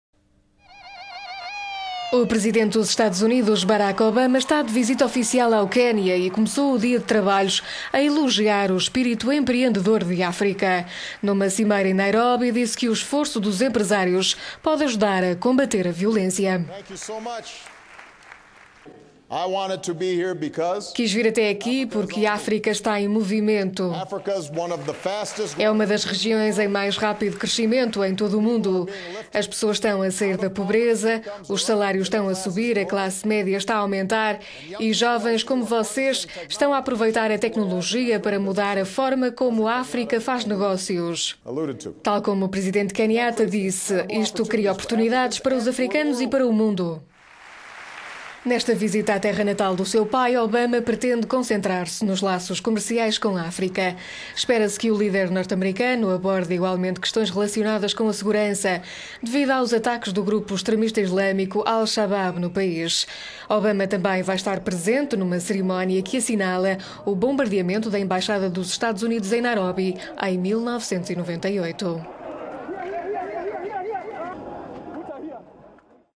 Obama de visita ao Quénia para estreitar laços comerciais
O presidente dos Estados Unidos, Barack Obama está de visita oficial ao Quénia e começou o dia de trabalhos a elogiar o espírito empreendedor de África.

Em discurso, numa cimeira em Nairobi, disse que o esforço dos empresários pode ajudar a combater a violência e a corrupção: "Quis vir até aqui, porque África está em movimento. É uma das regiões em mais rápido crescimento de todo o mundo. As pessoas estão a sair da pobreza, os salários estão a subir, a classe média está aumentar e jovens como v…